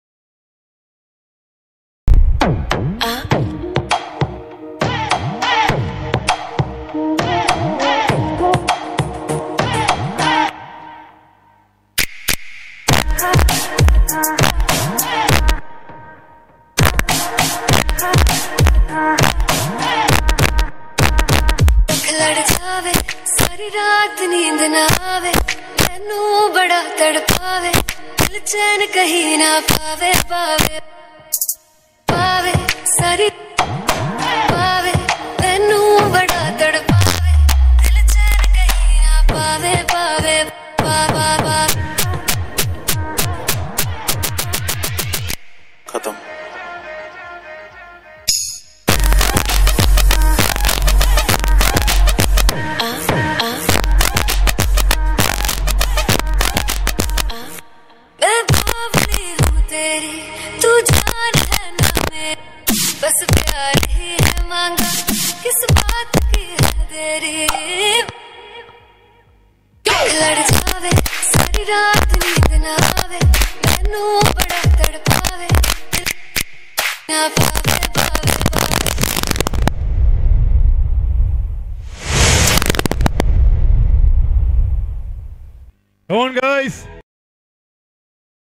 THIRAK - The Dance Champions Season 02 Second Round Dance Performance - 42 | Schools DNA
THIRAK - The Dance Champions Season 02 is a dance competition exclusively organised for ALWAR district dancers by SCHOOLS DNA.